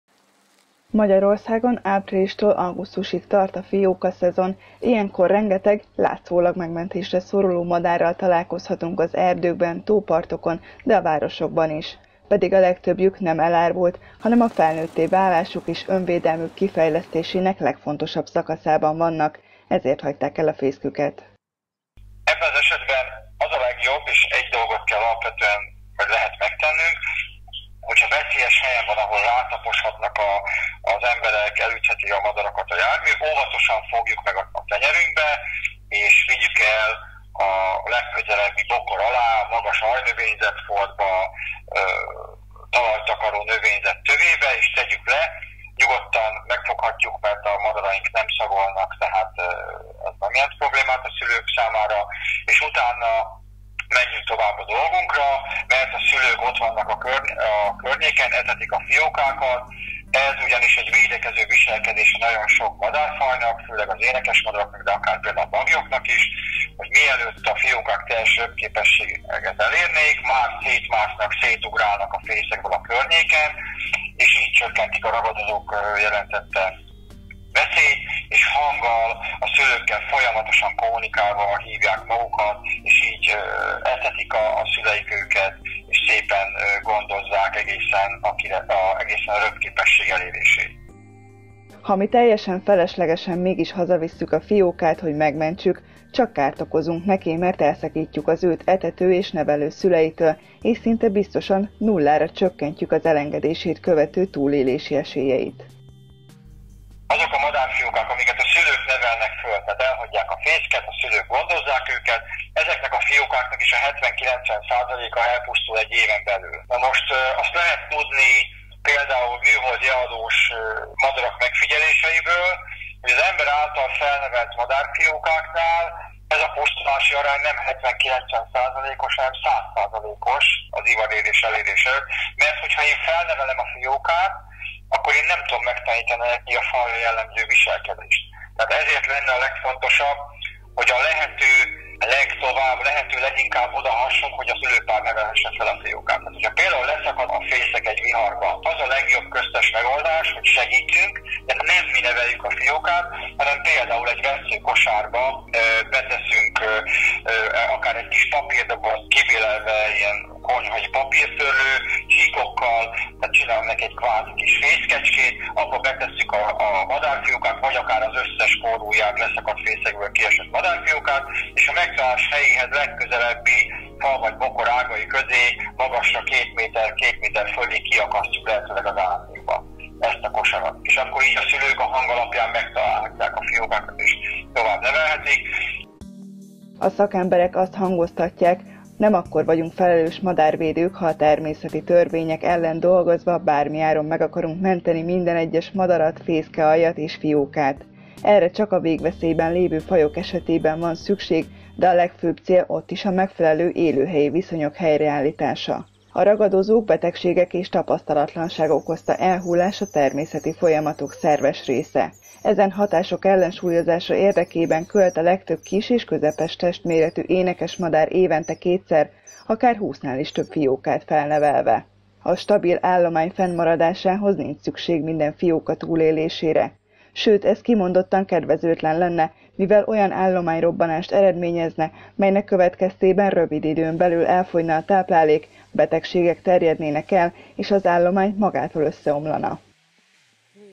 Ne vigyük haza a talált madárfiókát! - Kölcsey Televízió
A fiókák 70-80%-a mindenképpen elpusztul, ez a természet alaptörvénye. Ezért nem szabad a járdán vagy tóparton talált madarat hazavinni, akkor ugyanis közel 0% a túlélési esélyük – mondják a szakemberek. A magányosnak tűnő fióka ráadásul többnyire nem is árva.